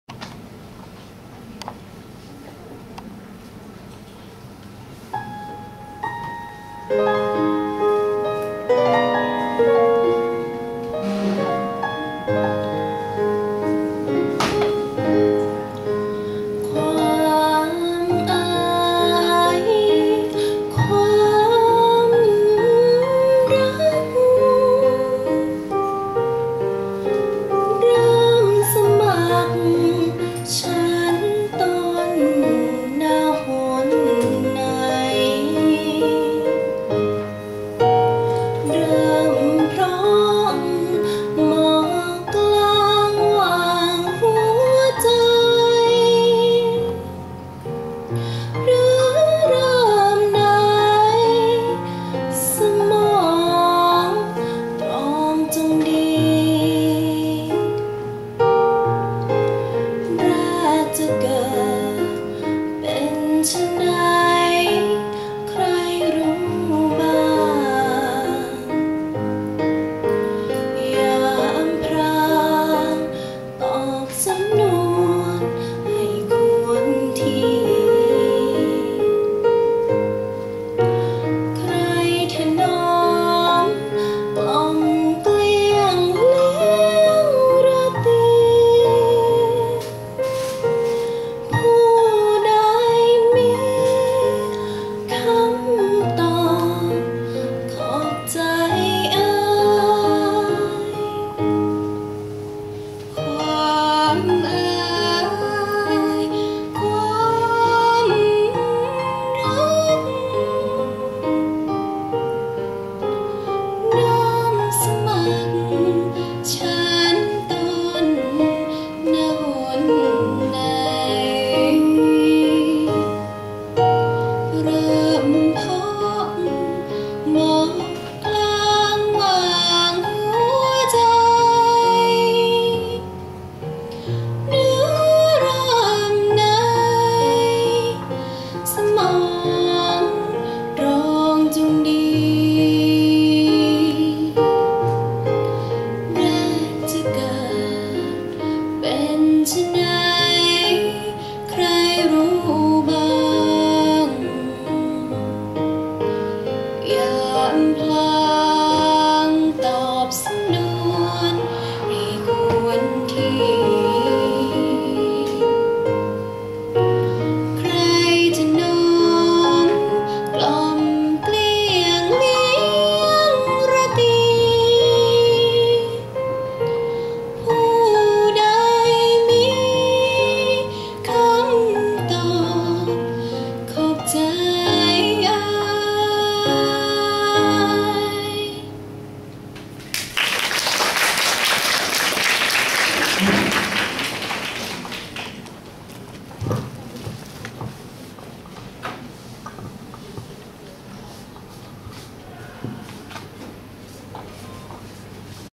"ความรัก" เบิกโรง ละครพูด รัชกาลที่6
เบิกโรง กิจกรรมการแสดงละครพูดของนักศึกษาในห้องเรียนวิชาวรรณคดีสมัยรัชกาลที่ 6 ของ รศ.กัญญรัตน์  เวชชศาสตร์  หรือ คณะละครสมัครเล่นทับแก้ว เรื่อง "เวนิชวานิช"  (พ.ศ.2557)